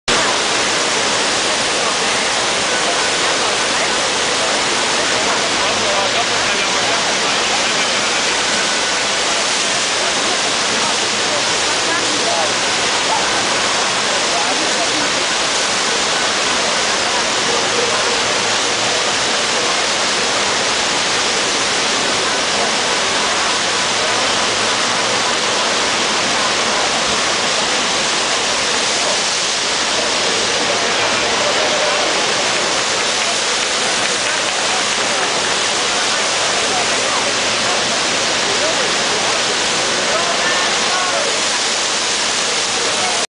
Finale del primo surf contest "King of Slippy" sulla nave da crociera "Independence of the Seas"
Video della finale del primo surf contest "King of Slippy" che si è tenuta sulla nave da crociera Independence of the Seas della Royal Caribbean International.